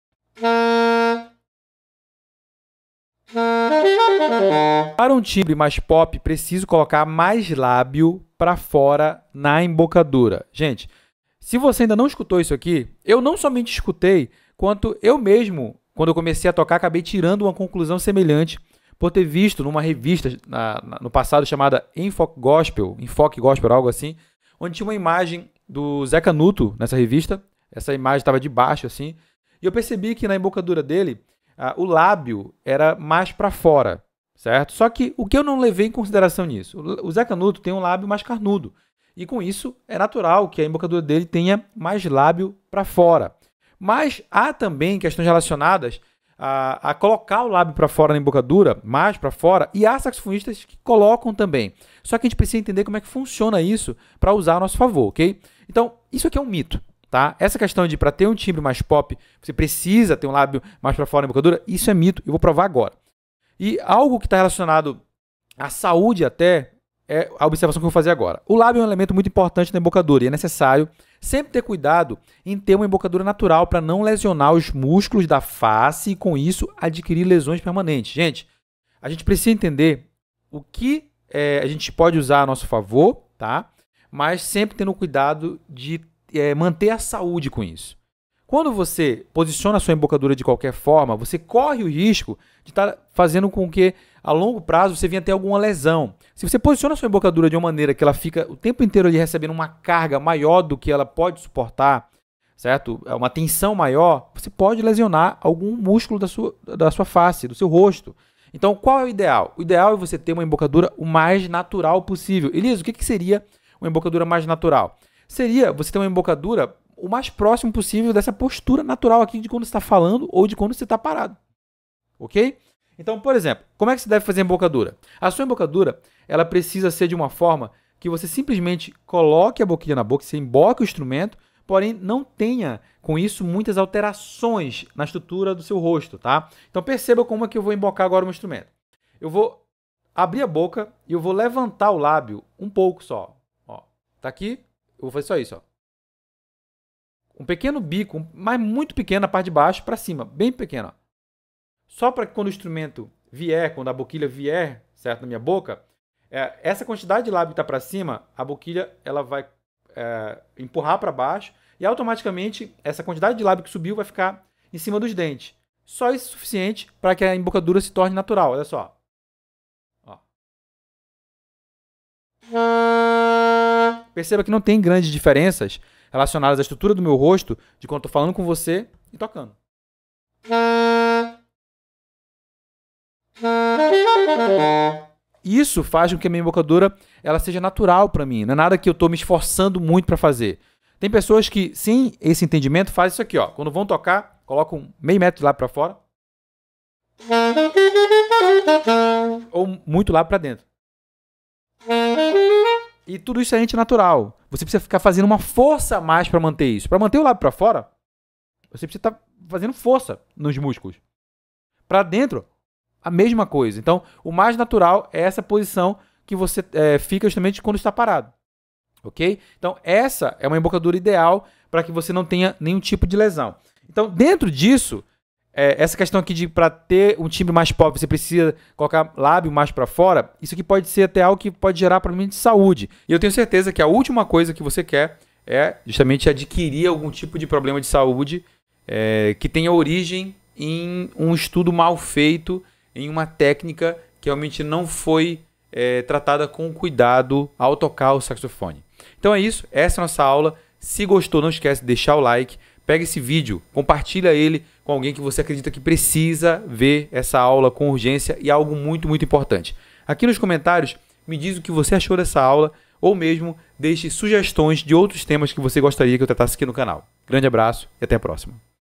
A Embocadura no Saxofone - Mitos Para Tocar Sax Pop | Elias Coutinho
É possível que você já tenha ouvido algo sobre o lábio precisar estar mais para fora ou mais para dentro na embocadura do saxofone. Muito do que ouvimos, não passam de mitos sem embasamento algum, por isso cuidado e siga as dicas que mostro nessa aula.